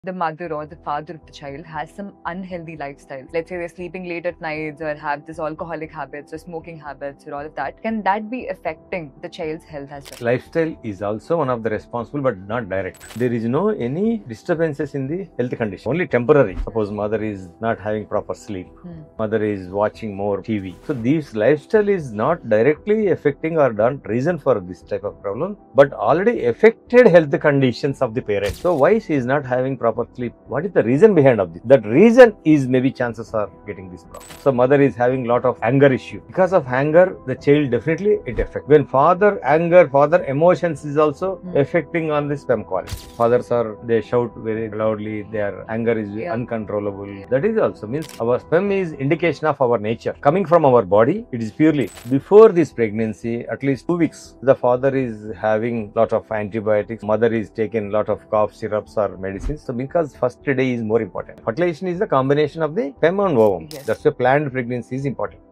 Will Lifestyle Be Responsible for Autism?
Does lifestyle cause autism?
Here’s the truth based on science.
Watch this to understand what actually influences autism and what doesn’t.